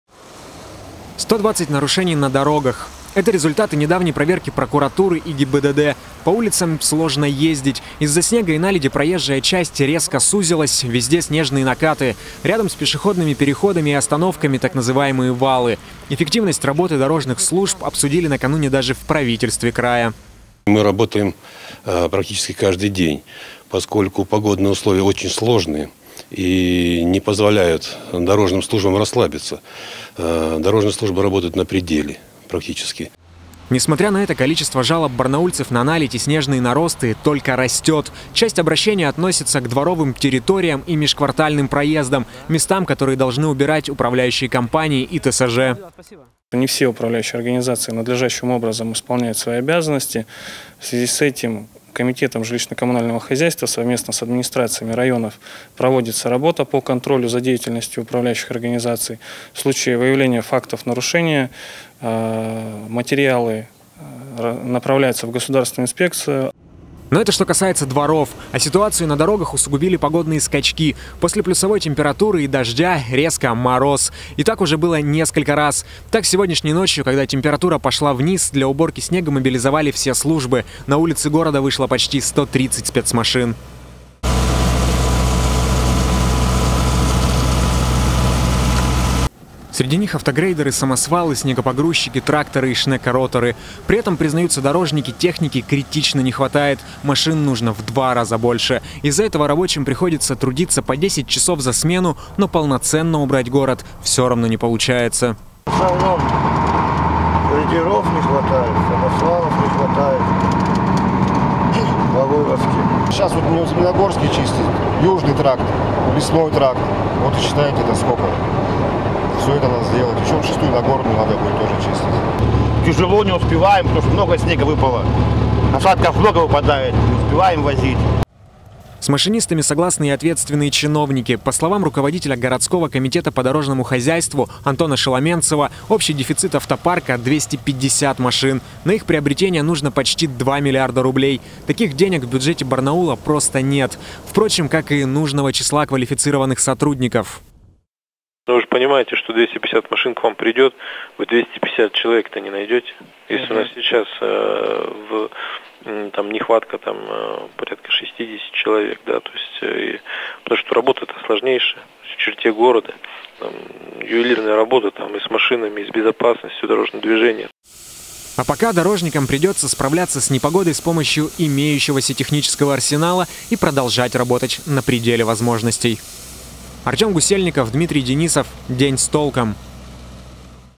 Резкая смена погодных условий и нехватка техники: как дорожные работники справляются со стихией
Проблема некачественной чистки барнаульских дорог не теряет своей актуальности. За последний месяц в адрес ответственных за это чиновников, что называется, камень не кинул только ленивый. А между тем, специалисты подчеркивают: виной тому несколько объективных причин. И обильные осадки вперемежку с резкими погодными колебаниями — не основная из них. Куда острее стоит вопрос с нехваткой снегоуборочной техники и кадров.  Мы решили на деле убедиться, каково это чистить зимой городские улицы,  и попытались выяснить, насколько правдивы  аргументы дорожников. Тему продолжит Артему Гусельников